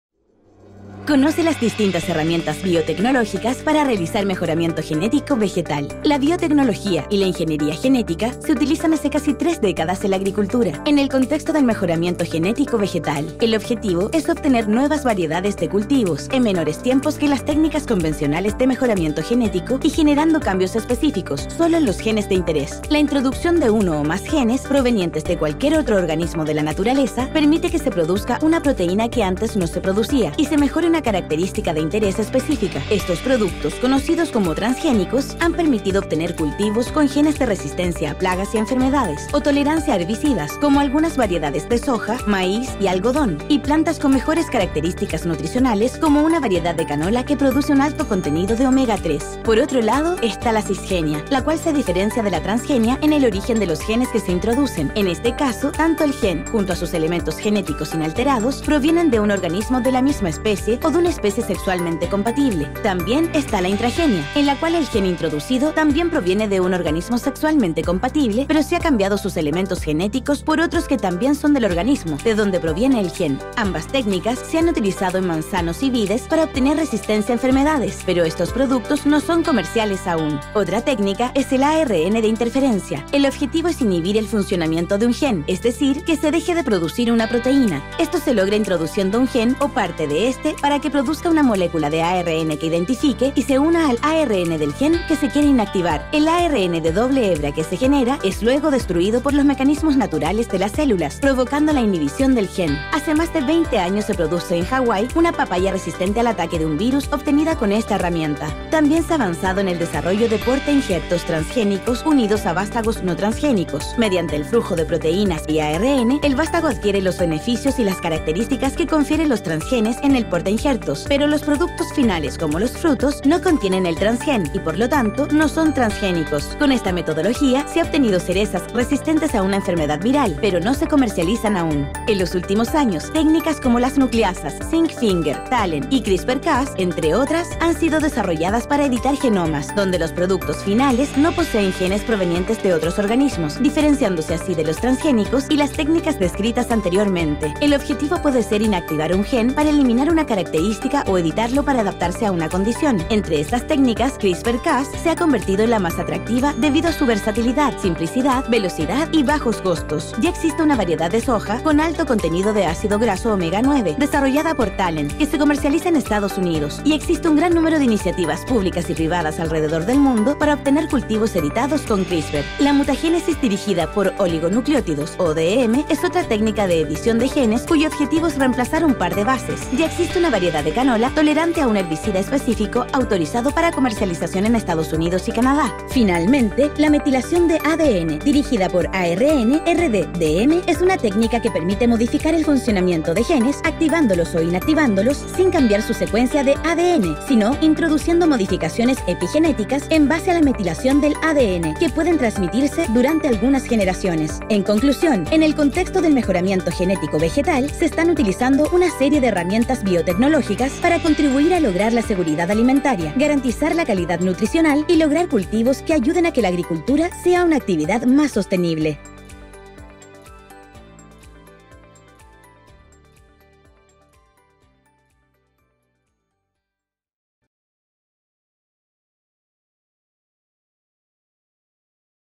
Conoce las distintas herramientas biotecnológicas para realizar mejoramiento genético vegetal
Infórmate en chilebio.cl